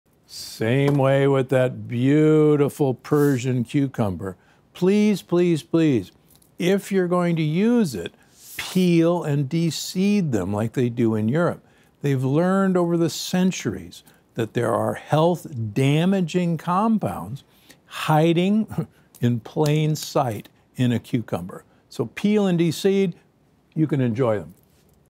Why You Should Peel and Deseed Cucumbers Before Eating: Tips for Healthy Snacking
We'll be talking about cucumbers and why it's important to peel and deseed them before eating. While cucumbers are a healthy and refreshing snack, they can also be difficult to digest if not prepared properly.